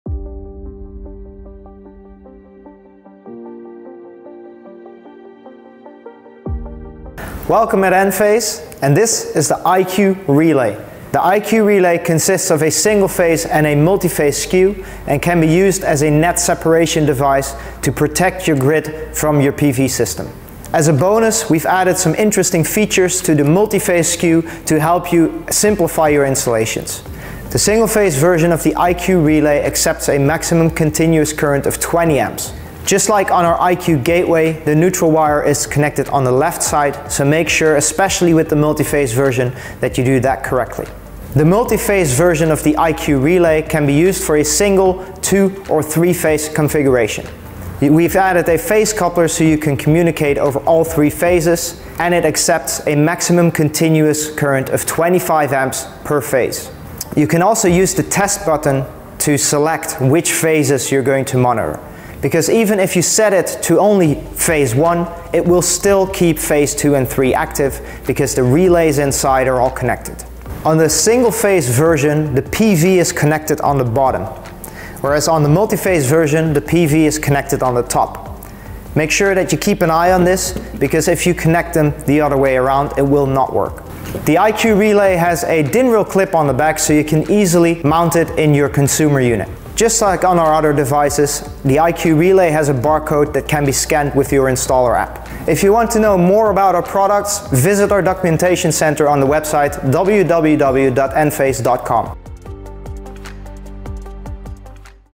Enphase - Introduction IQ Relay (English)
This video provides more information about the IQ Relay. In Enphase installations, the IQ Relay functions as a network monitoring and decoupling device and includes a built-in phase coupler for distributing communication signals in power lines between phases.